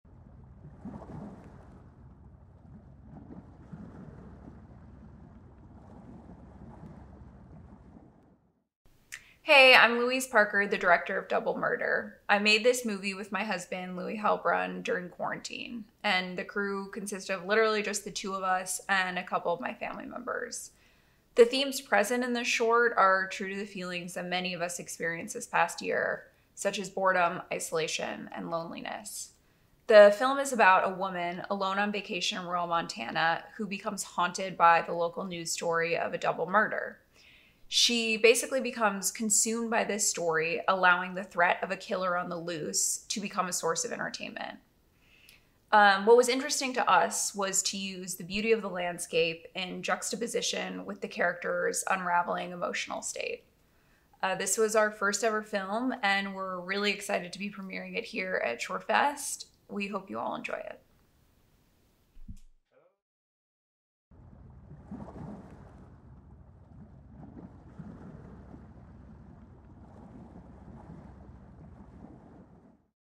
ShortShots 2021: Double Murder
Louise Parker, co-writer, producer, and director of Double Murder.

Alone on vacation in rural Montana, a woman becomes haunted by the local news story of a double murder.

In competition for Best U.S. Short.

This short is a part of the In the Country program, screening 6/23 at 1:45PM.